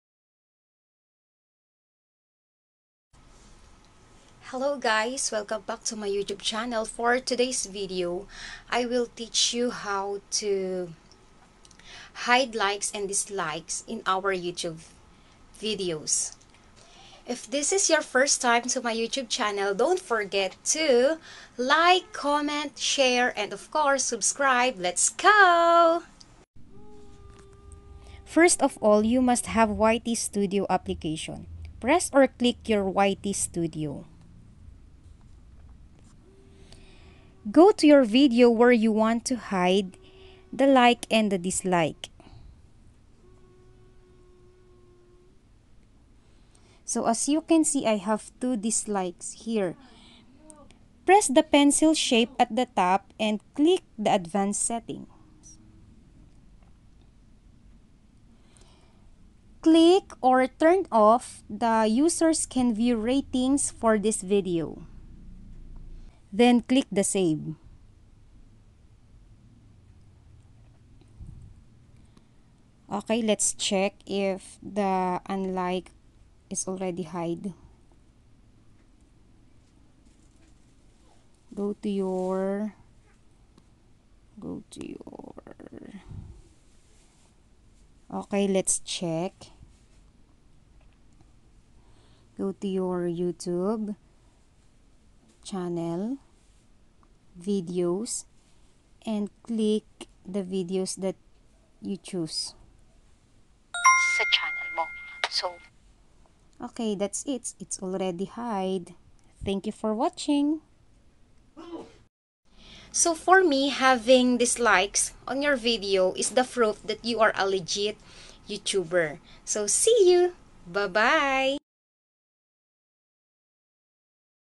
HowTo HideLikesAndDislikes
How to hide like and dislike from your yt Videos/ Paano mag hide ng Like at dislike ng video mo sa yt
hide like and dislike